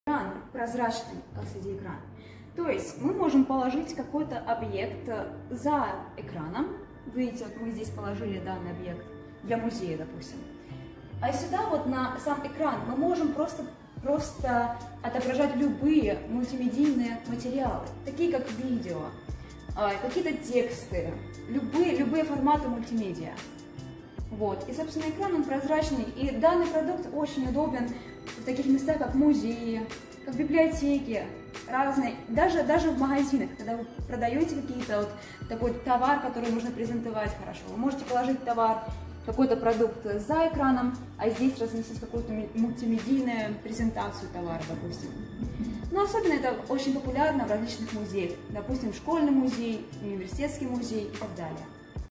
💠Прозрачные дисплеи — OLED: Создание Музейных Витрин💠
OLED -- это дисплеи на основе органических светодиодов. Эти экраны могут самостоятельно излучать свет, поэтому не нуждаются в дополнительной подсветке.
💠Отдельно стоящие прозрачные дисплеи позволяют создавать локальные инсталляции объединяющие физические предметы и контент;
💠Дисплеи для установки на витринное стекло усиливают воздействие рекламной;
💠Гибкие пленочные прозрачные OLED дисплеи позволяют сделать прозрачный дисплей на криволинейной поверхности.

⭐Еще одним применением прозрачных OLED дисплеев является создание музейных витрин, которые объединяют функции демонстрации музейного экспоната и предоставления информации о нем на дисплее.

⭐CreateView Edu-Tech является одним из передовых производителей прозрачных OLED дисплеев в Китае. Доставляем в русскоязычные страны из Гуанчжоу.